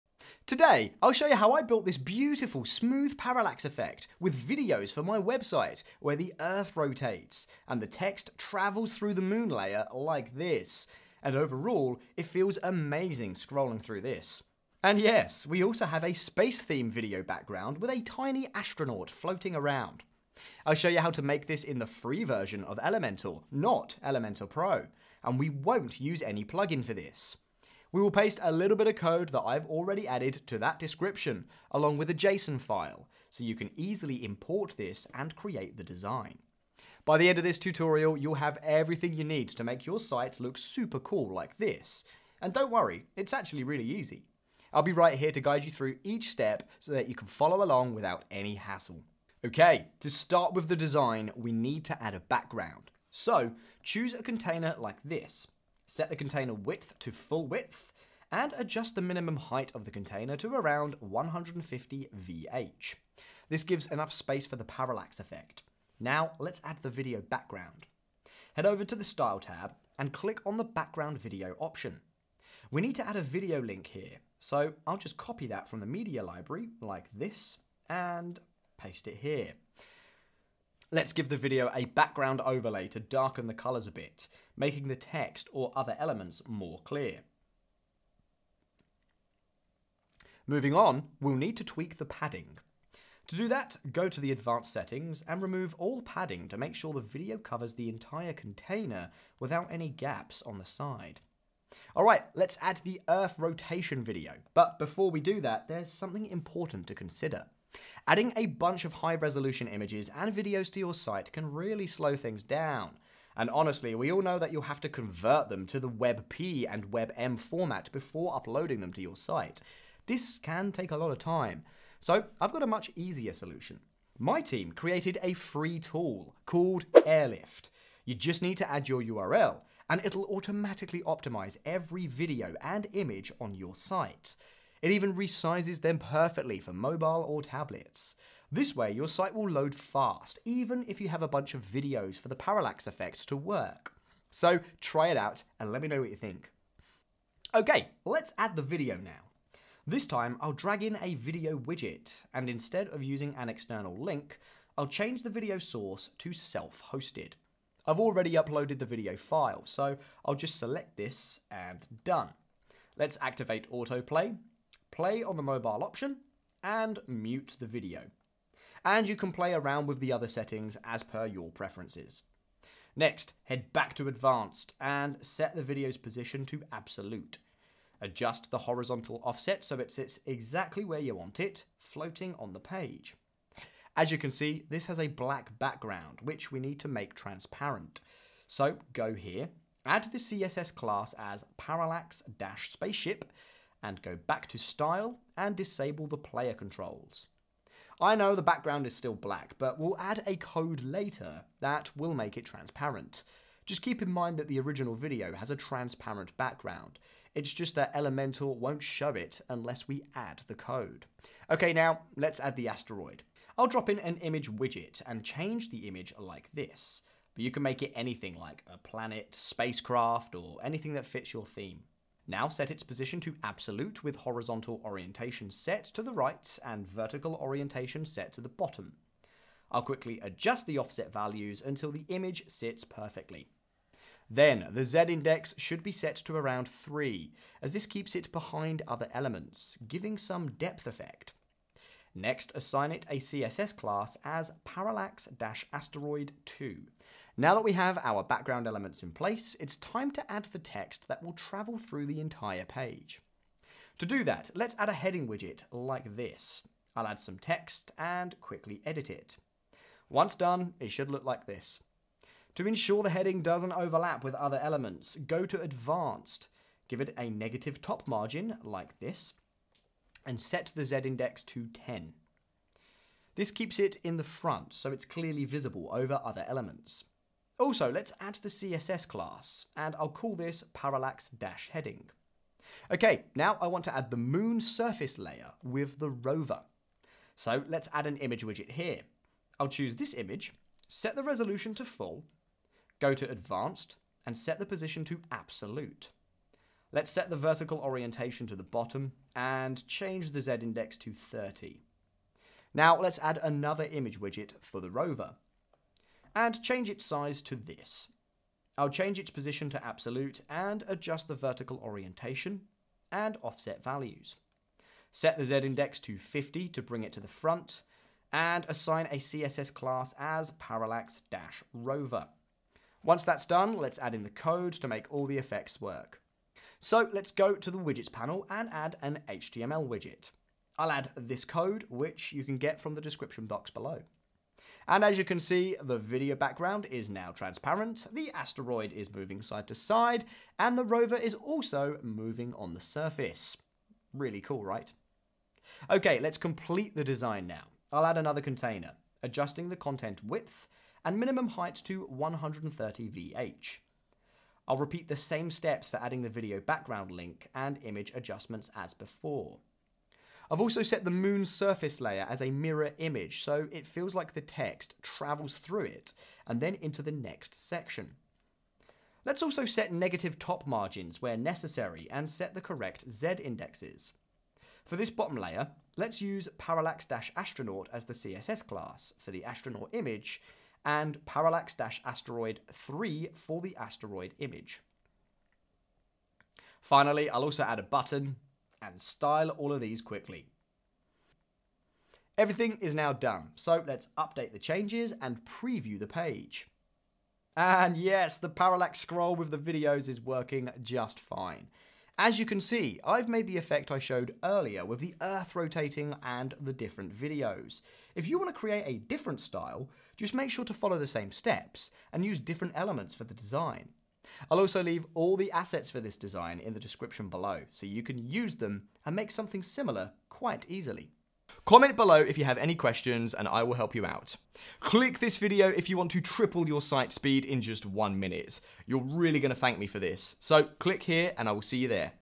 First EVER Video Parallax Effect (Elementor)
⭐ Giveaway: Our 100% FREE plugin makes any site 3X faster In this video, I’ll show you how to create an incredible parallax scrolling effect with videos for your website — complete with a rotating earth, a moon layer with text traveling through, and even a floating astronaut in a space-themed video background. The best part? You can do this in the free version of Elementor without any plugins, just by adding a bit of custom code and a JSON file (linked in the description).

I’ll guide you step-by-step to design this smooth, surreal scrolling experience. From setting up video backgrounds and layering elements like asteroids and moon surfaces to customizing CSS classes for animations — everything is covered. By the end, you’ll have all the tools to make your website stand out with this stunning effect. Let’s get started!

00:00 Create a video parallax scroll like this
00:49 Use a video background
03:08 Add the CSS classes
04:09  How to create the heading scroll
05:22 Adding HTML Code (don't miss!)
06:35 Final preview